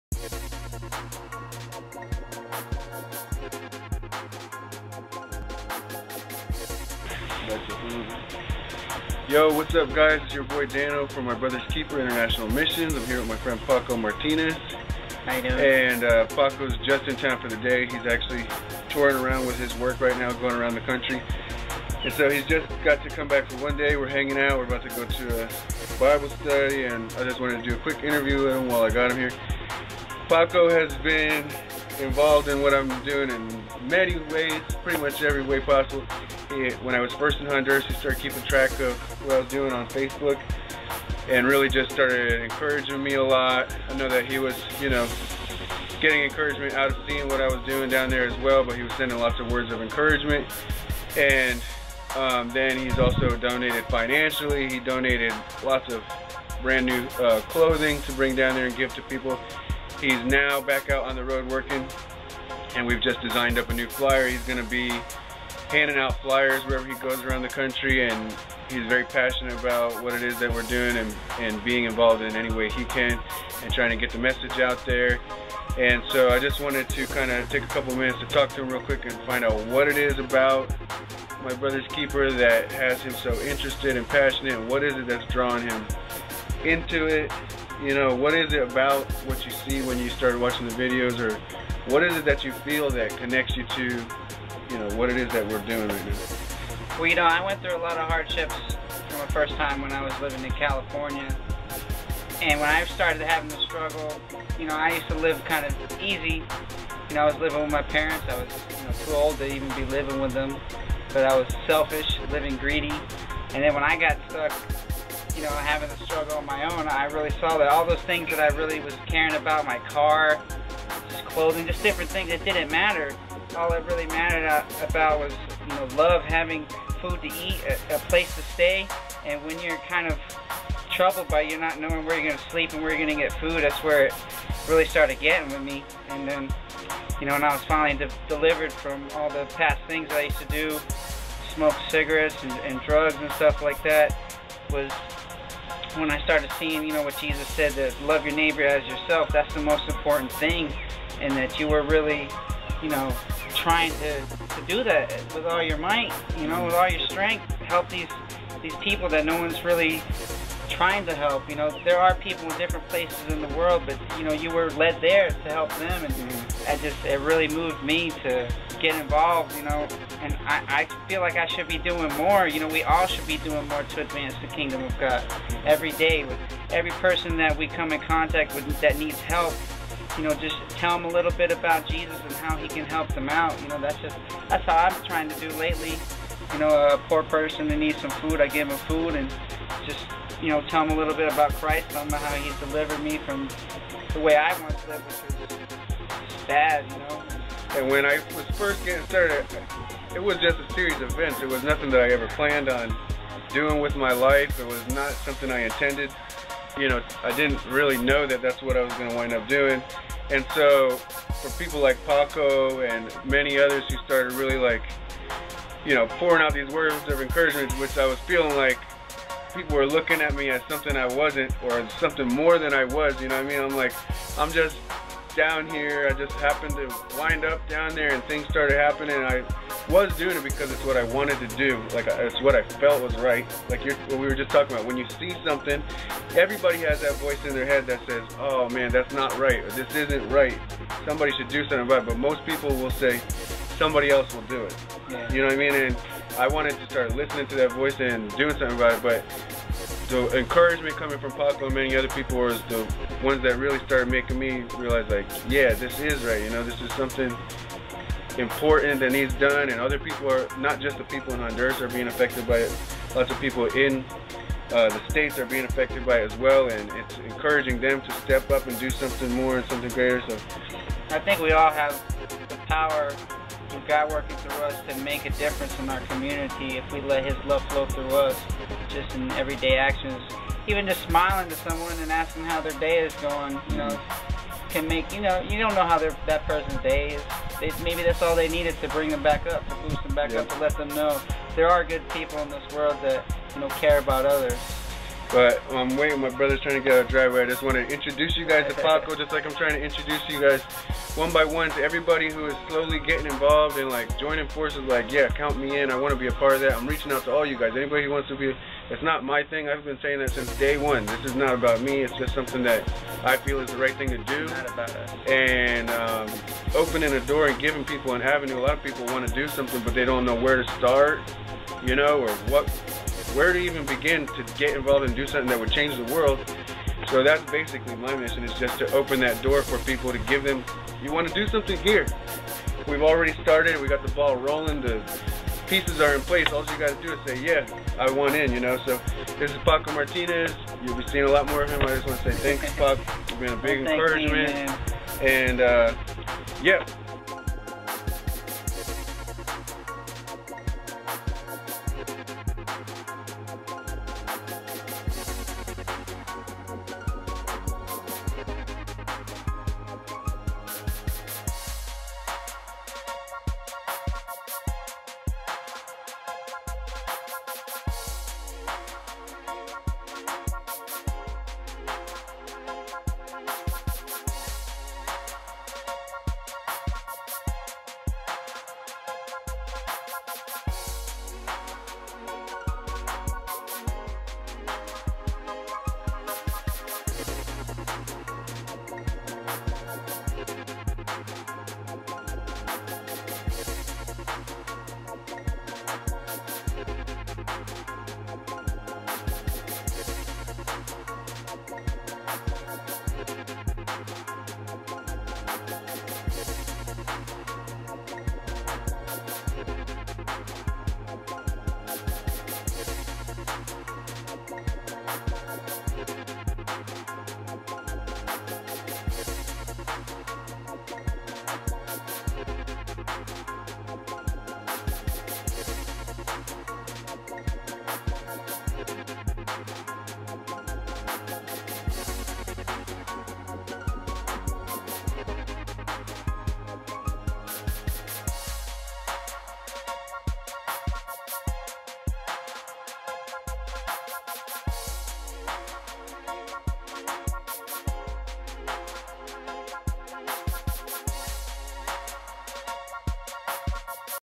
Volunteer Spotlight: Paco Martinez
Special thanks to my friend and co-laborer in the mission to bring Love, Hope, and Change to the people of Honduras- Fransisco "Paco" Martinez. Paco has contributed financially, Donated clothing, been passionately spreading the word, and plans to come visi...t us in Honduras in the near future. He has been a huge encouragement and YOU TOO can experience the JOY and SATISFACTION that he is feeling as a result of his giving of himself to a worthy cause.